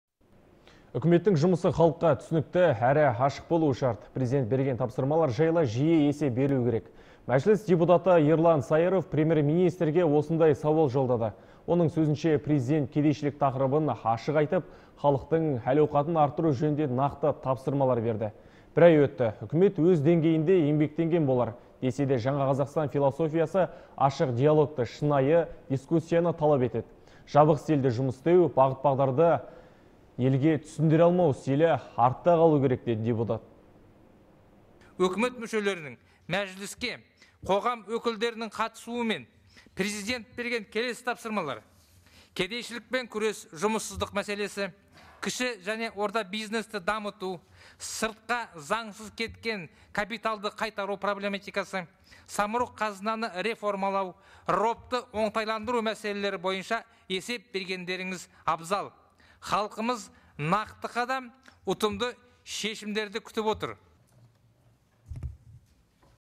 Үкіметтің жұмысы халыққа түсінікті, әрі ашық болуы шарт
Үкіметтің жұмысы халыққа түсінікті, әрі ашық болуы шарт. Президент берген тапсырмалар жайлы жиі есеп берілу керек. Мәжіліс депутаты Ерлан Сайыров Премьер министрге осындай сауал жолдады! Оның сөзінше Президент кедейшілік тақырыбын ашық айтып, халықтың әл-ауқатын арттыру жөнінде нақты тапсырмалар берді. Бір ай өтті, Үкімет өз деңгейінде еңбектенген болар, десе де «Жаңа Қазақстан» философиясы ашық диалогты, шынайы дискуссияны талап етеді. Жабық стилде жұмыс істеу, бағыт бағдарды елге түсіндіре алмау стилі артта қалу керек деді депутат.